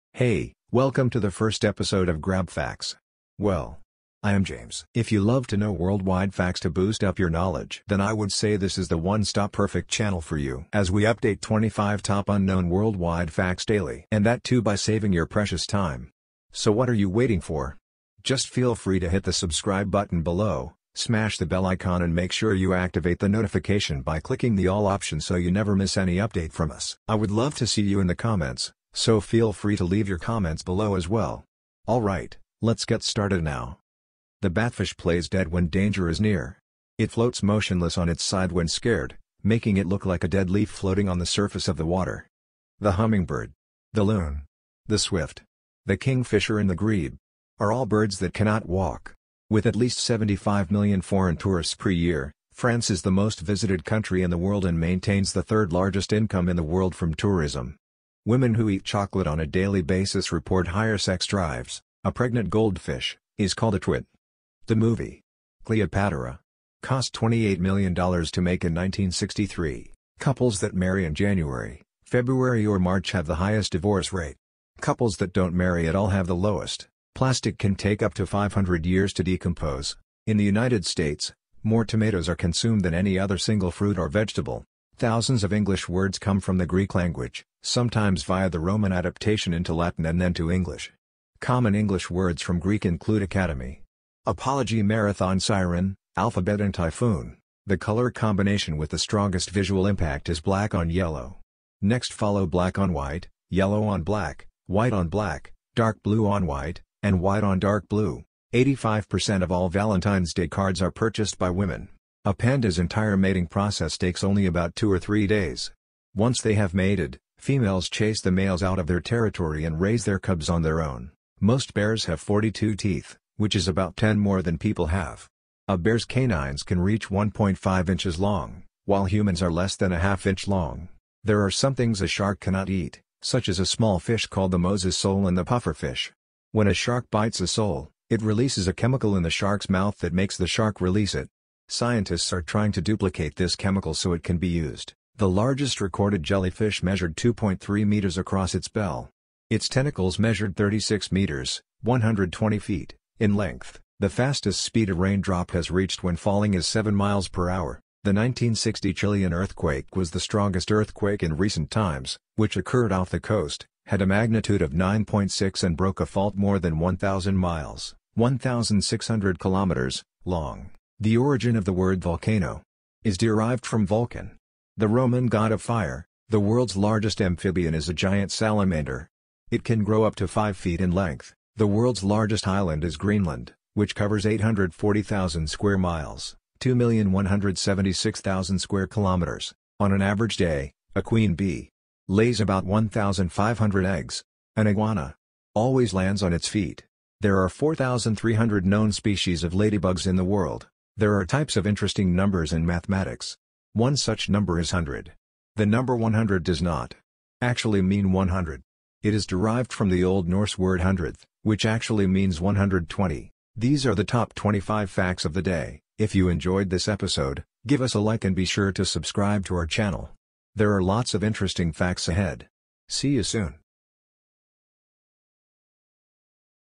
TOP 25 QUICK FACTS of the day in English || Have a quick look || GrabFacts
These are the top 25 Facts of the day covered in the video and the same have been mentioned here in jumble way :-

1) The world's largest island is Greenland, which covers 840,000 square miles (2,176,000 square kilometres)

2) On an average day, a queen bee lays about 1,500 eggs.

3) An iguana always lands on its feet.

4) There are 4,300 known species of ladybugs in the world.

5) There are types of interesting numbers in mathematics. One such number is hundred. The number 100 does not actually mean 100. It is derived from the Old Norse word hundredth, which actually means 120.

6) The fastest speeda raindrop has reached when falling is seven miles per hour.

7) The 1960 Chilean earthquake was the strongest earthquake in recent times, which occurred off the coast, had a magnitude of 9.6 and broke a fault more than 1000 miles (1600 kilometres) long.

8) The world's largest amphibian is a giant salamander. It can grow up to 5 ft. in length.

9) The origin of the word volcano is derived from Vulcan, the Roman god of fire.

10) The largest recorded jellyfish measured 2.3 m across its bell. Its tentacles measured 36 m (120 feet) in length.

11) There are some things a shark cannot eat, such as a small fish called the Moses sole and the pufferfish. When a shark bites a sole, it releases a chemical in the shark's mouth that makes the shark release it. Scientists are trying to duplicate this chemical so it can be used.

12) Most bears have 42 teeth, which is about 10 more than people have. A bear's canines can reach 1.5" long, while humans are less than a half-inch long.

13) A panda's entire mating process takes only about two or three days. Once they have mated, females chase the males out of their territory and raise their cubs on their own.

14) The batfish plays dead when danger is near. It floats motionless on its side when scared, making it look like a dead leaf floating on the surface of the water.

15) 85% of all Valentine's Day cards are purchased by women!.

16) The hummingbird, the loon, the swift, the kingfisher, and the grebe are all birds that cannot Walk.

17) The color combination with the strongest visual impact is black on yellow. Next follow black on white, yellow on black, white on black, dark blue on white, and white on dark blue.

18) The movie Cleopatra cost $28 million to make in 1963.

19) A pregnant goldfish, is called a 'twit'.

20) Women who eat chocolate on a daily basis report higher sex drives.

21) With at least 75 million foreign tourists per year, France is the most visited country in the world and maintains the third Largest income in the world from tourism.

22) Thousands of English words come from the Greek language, sometimes via the Roman adaptation into Latin and then to English. Common English words from Greek include "academy." "apology" "marathon" "siren," "alphabet" and "typhoon.

23) In the United States, more tomatoes are consumed than any other single fruit or vegetable!.

24) Plastic can take up to 500 years to decompose.

25) Couples that marry in January, February or March have the highest divorce rate. Couples that dont marry at all have the lowest.

Other Images : Pixabay

Footage : Pexels.com
                  Pixabay.com
                  Youtube common creative licence